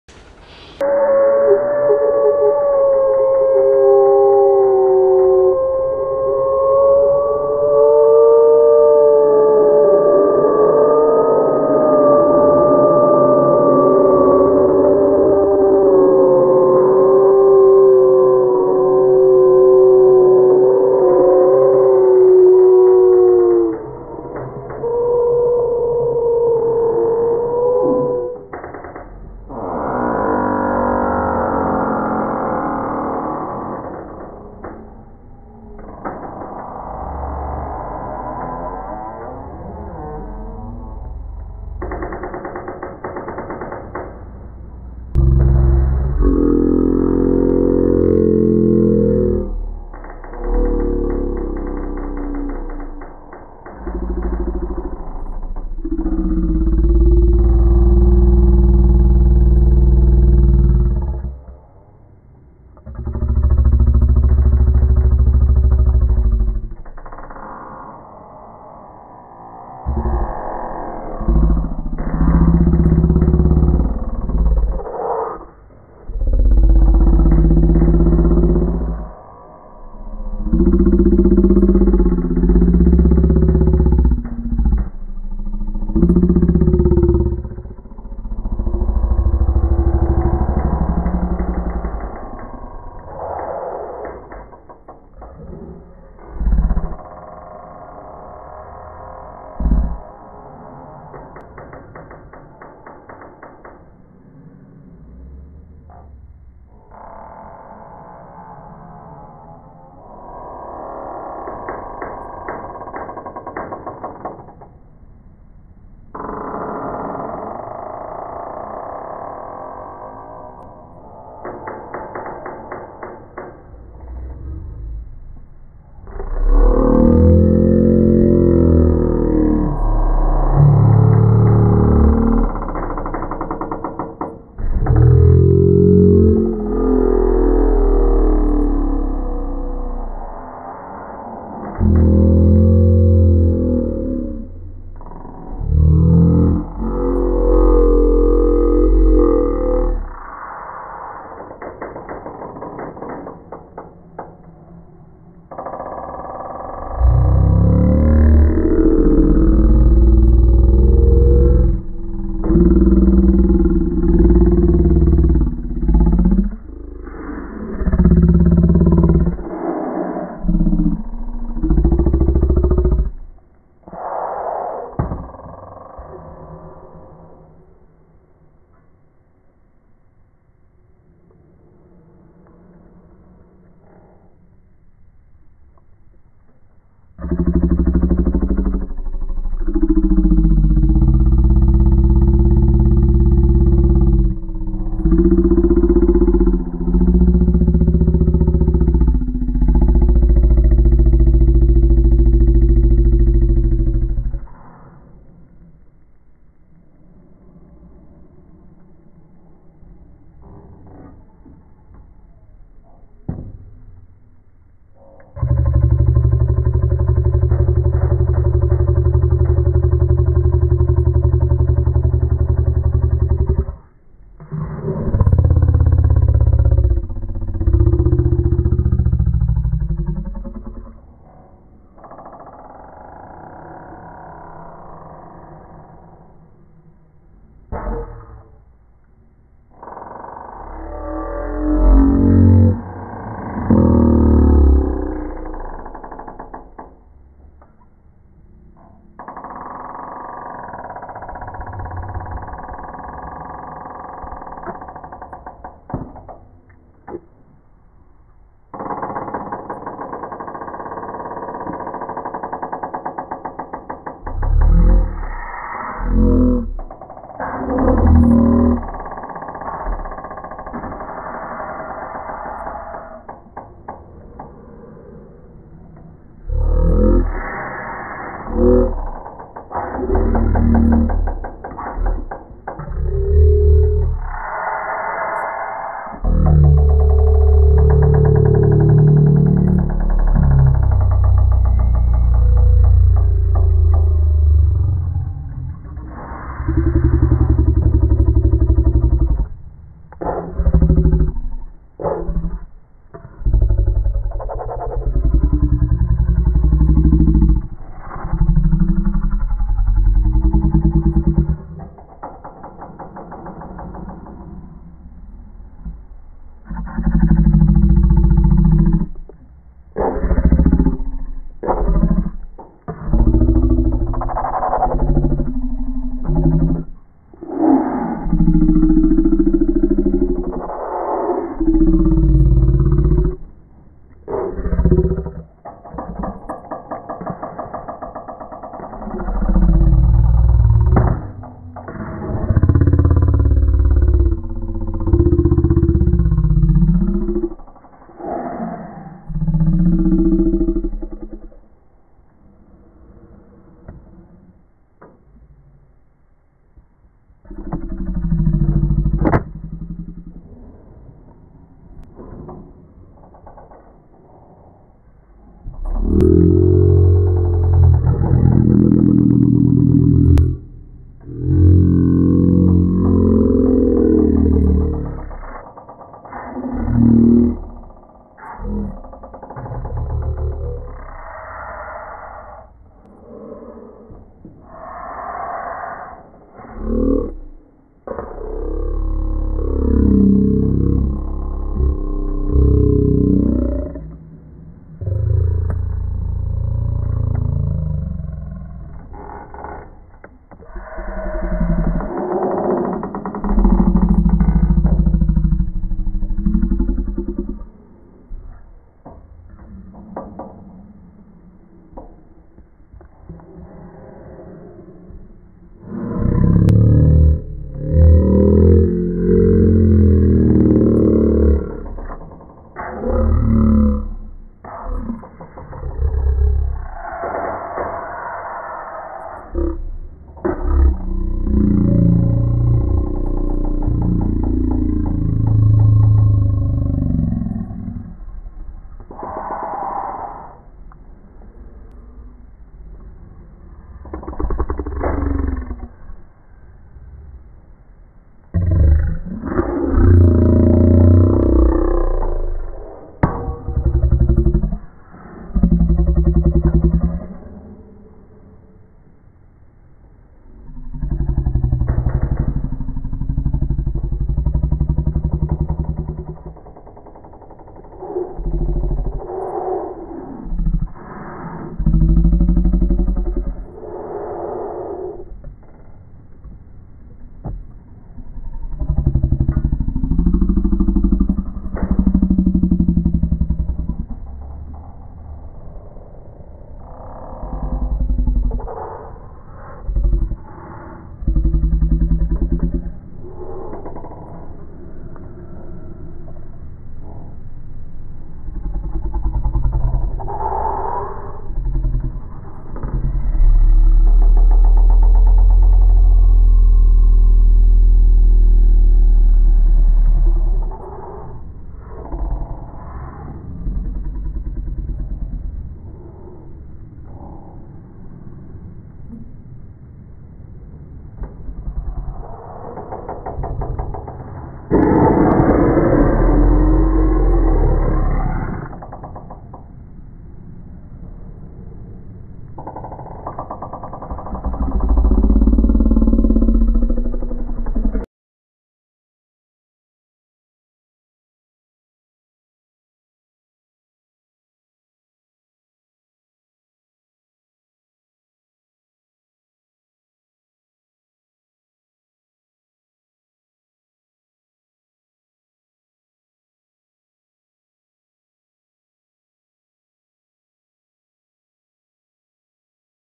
Russell & Ashley
This is a longer version, of the piece I have on my website. I find it amazing what slowing down a moment in time can do to an innocent game on the swings. Creating a demonic overtone to an already  scary baby sitting experience.